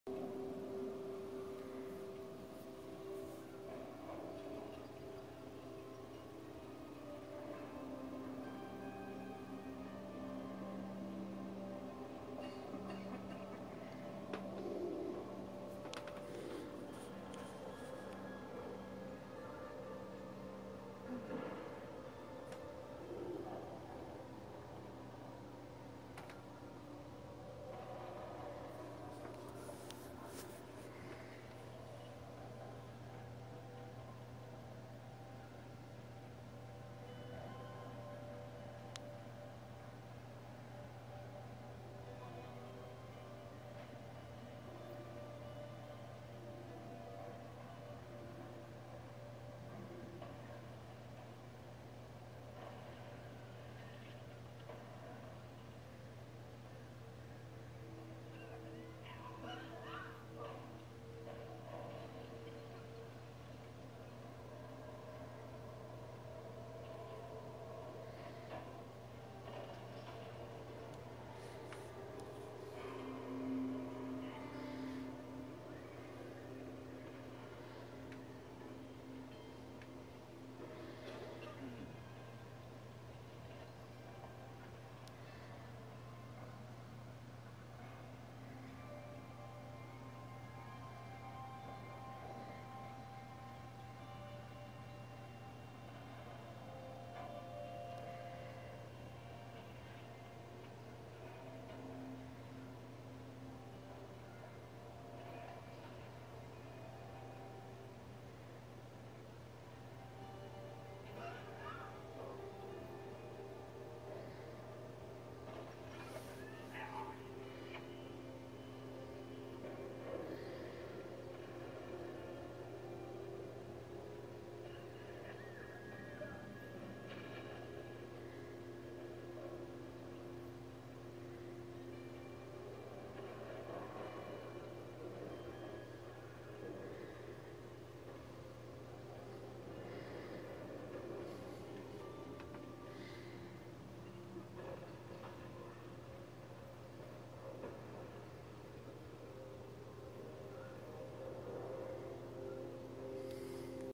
Ivan explores his new home
Ivan is my new pinkie tarantula exploring his new home. He was very stressed out at the pet shop (notice his bald abdomen) hopefully he is a little happier now that he can climb and wont have kids tapping on the glass all day long.